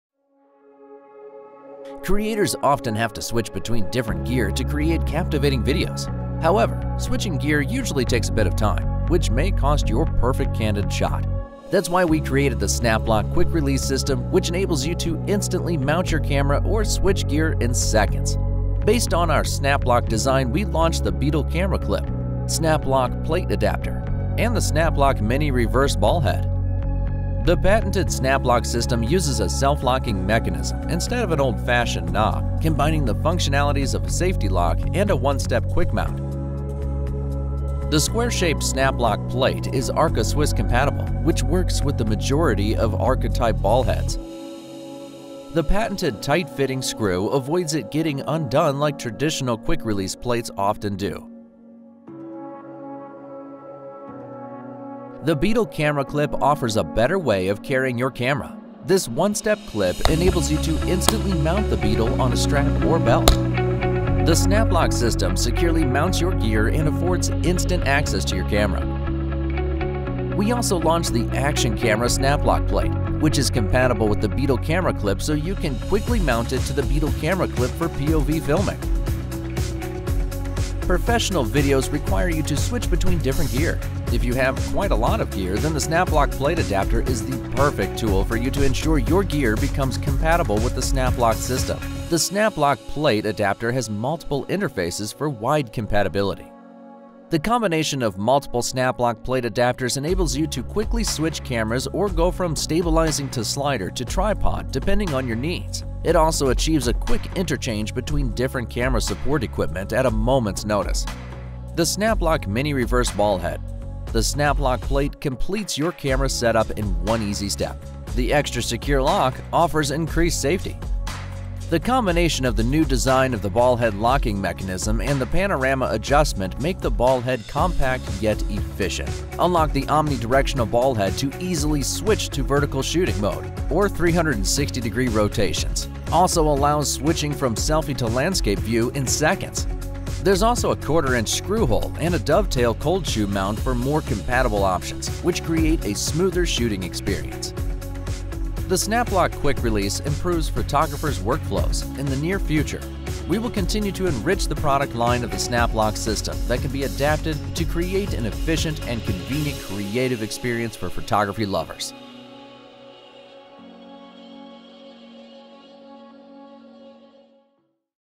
Introducing PGYTECH SnapLock Quick Release System
Introducing pgytech SnapLock Quick Release System. Includes the Beetle Camera Clip, SnapLock Plate Adapter, SnapLock Nano Swivel and Tilt Mount, Action Camera SnapLock Plate and the SnapLock MINI Reverse Ball Head. We created the SnapLock Quick Release System to mount and dismount your gears in seconds so that you can save your valuable time for content creation!